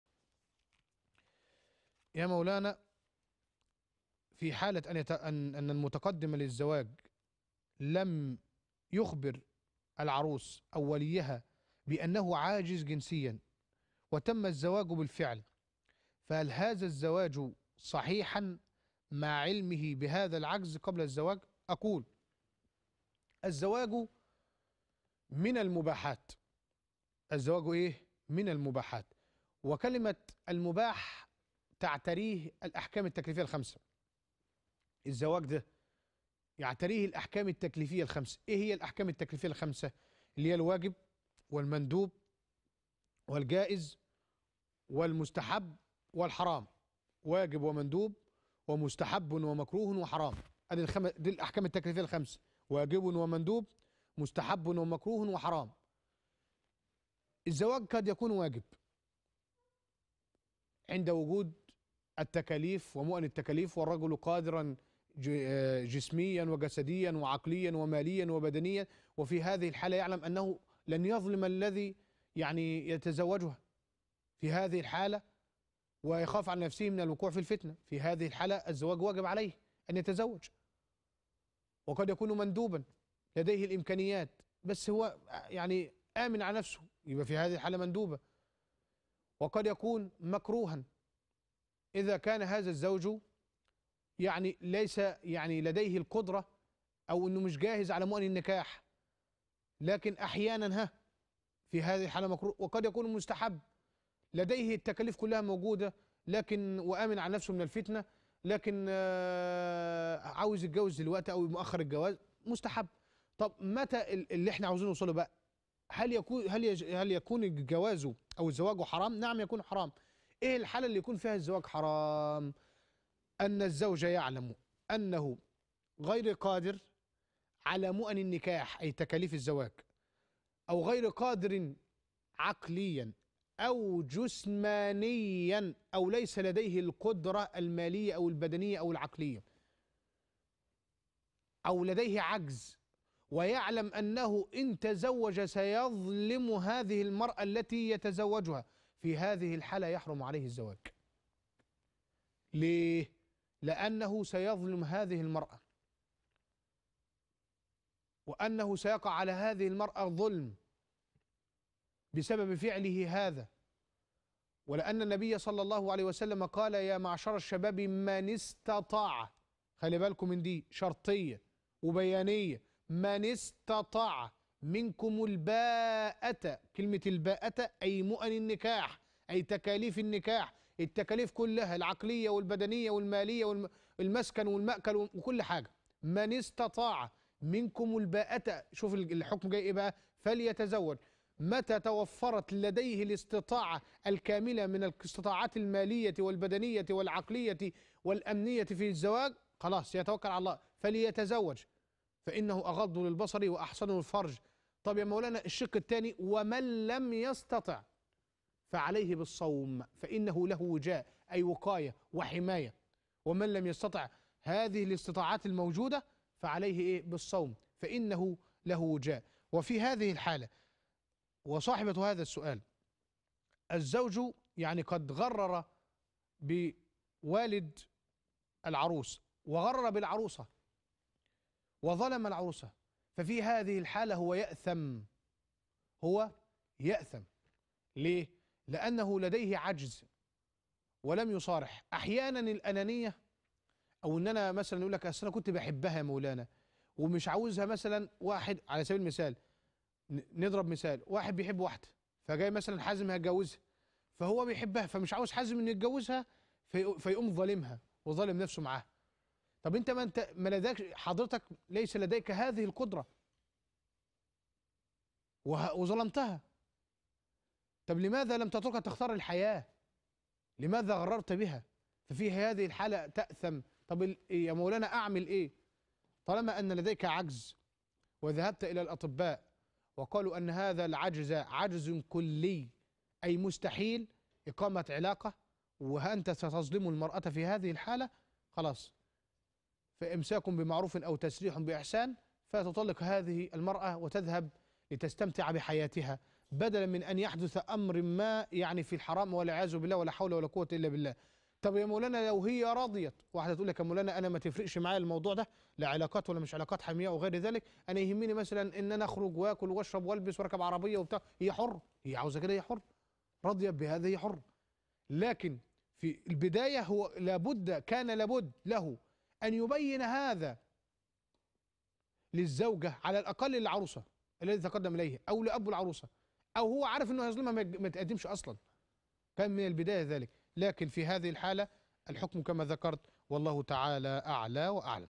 كيفية التصرف مع الزوج العاجز جنسيا - الشيخ حازم جلال
قناة المحور
موقع قناة المحورالالكتروني الرسمي